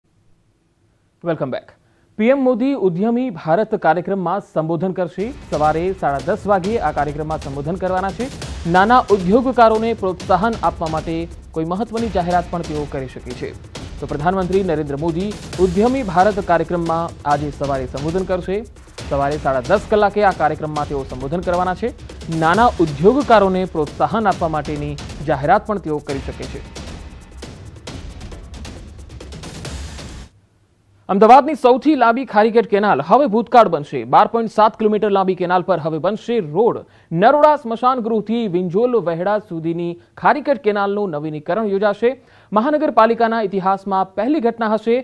PM Modi: લઘુ ઉદ્યોગોને પ્રોત્સાહન આપવા માટે પ્રધાનમંત્રીનું સંબોધન | News18 Gujarati
"ઉધમી ભારત" કાર્યક્રમને PM Modiનું સંબોધન, સવારે 10-30 કલાકે કરશે સંબોધન. આ સંબોધન દરમિયાન મહત્વની જાહેરાતો પણ PM Modi કરી શકે.